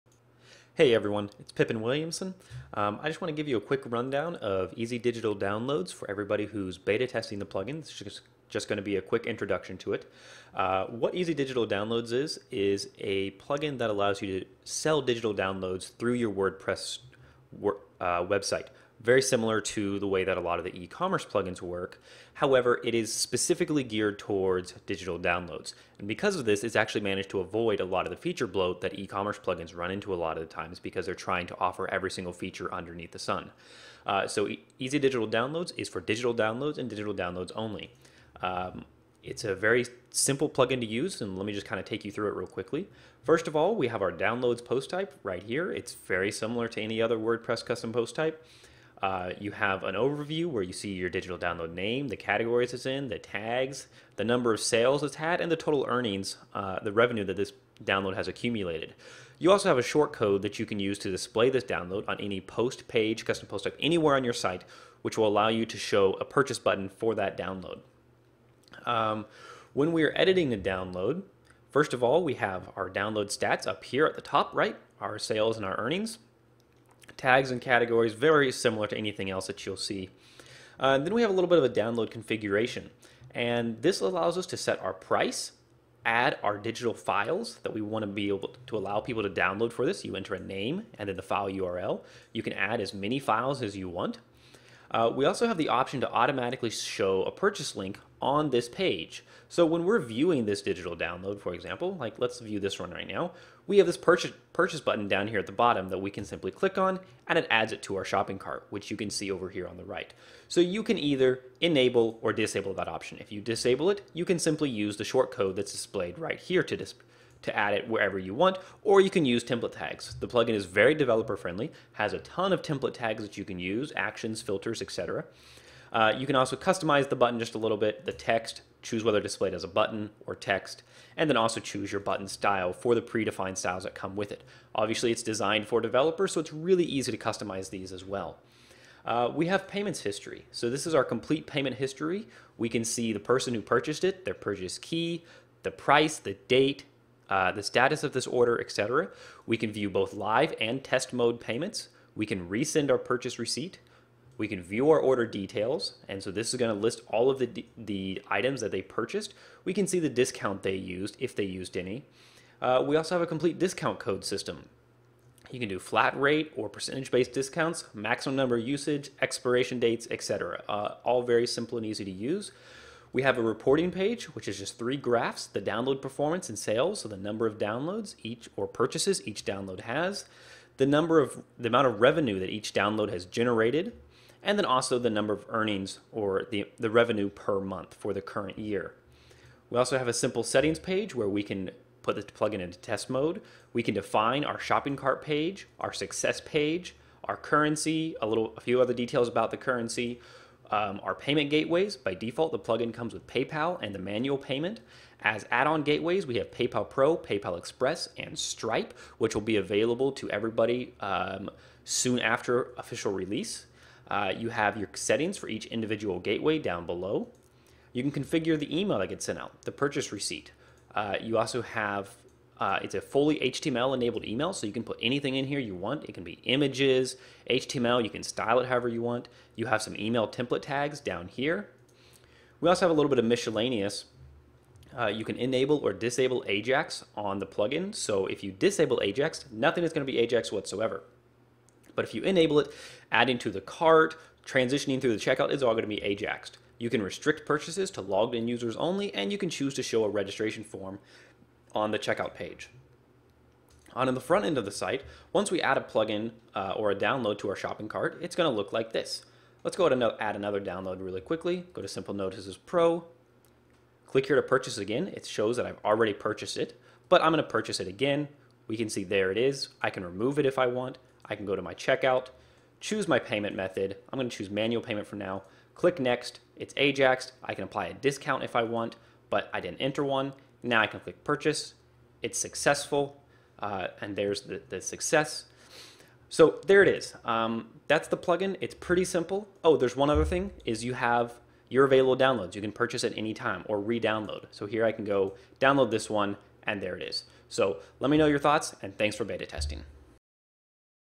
Introduction to Easy Digital Downloads for Beta testers
An introduction to Easy Digital Downloads for all beta testers. Not sure what it is yet? Watch the video!